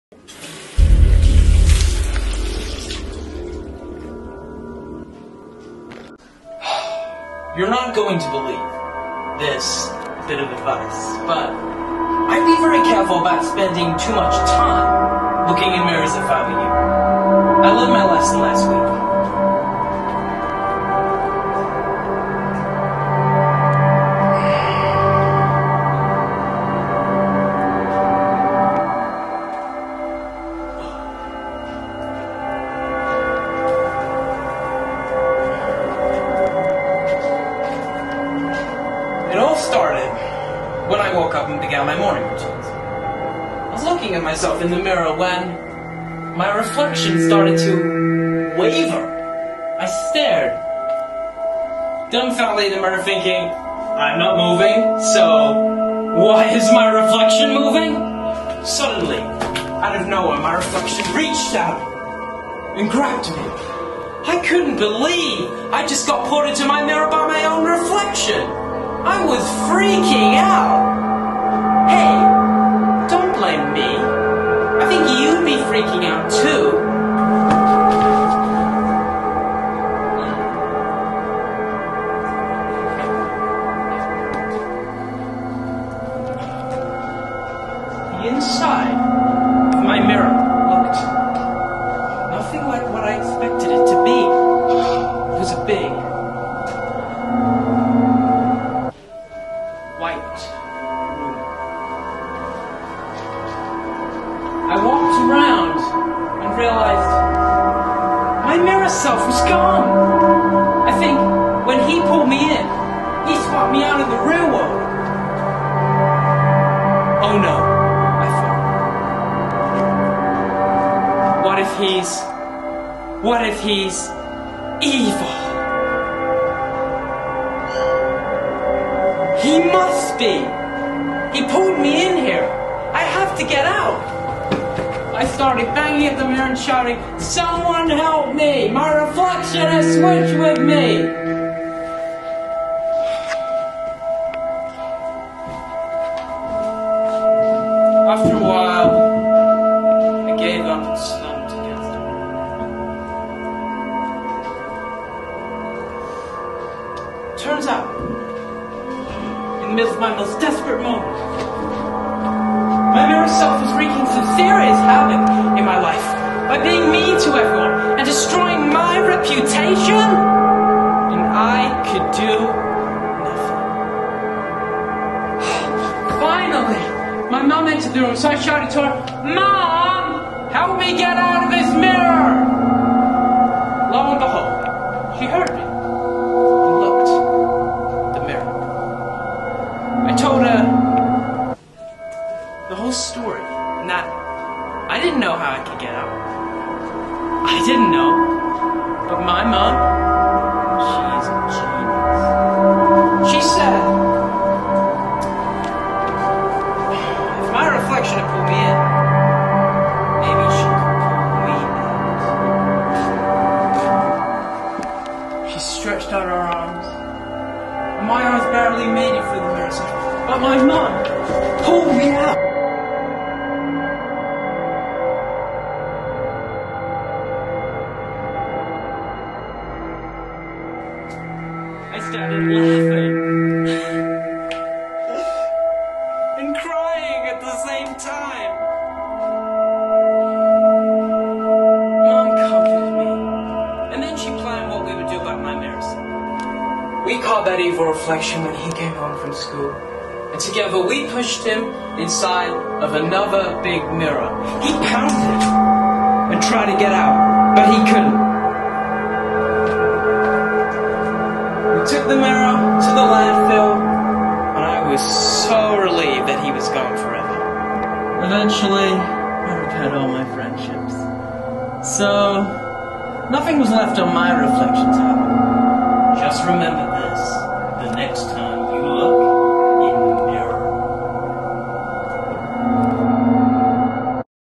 Evil Reflection Monologue
Hey People,

From the UAL Level 2 Diploma in Performing Arts Course, this is my FMP Film of my Evil Reflection Monologue. This was edited later by my peer.

My Crew:

.Man - Jade Patrick
.Filmer - Maddie Mcguire
.Editor - Maddie Mcguire

See if you can see them and tell me what you think of the film?

If you have any questions just let me know below...

~JP.